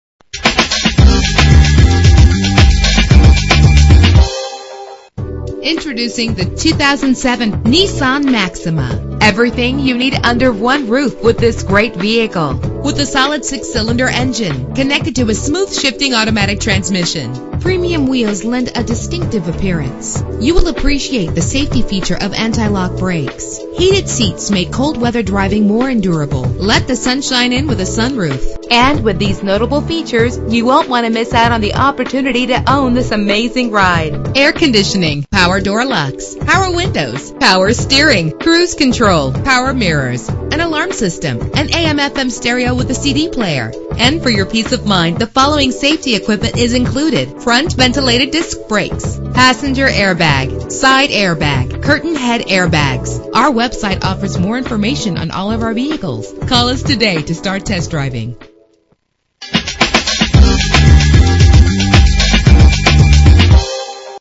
2007 Nissan Maxima - Hoffman Estates IL
Year: 2007
Make: Nissan
Model: Maxima
Trim: 3.5 SE
Engine: 3.5 liter 6 cylinder 24 valve
Transmission: Automatic CVT
Color: Black
Mileage: 70185
Address:
1099 W Higgins Rd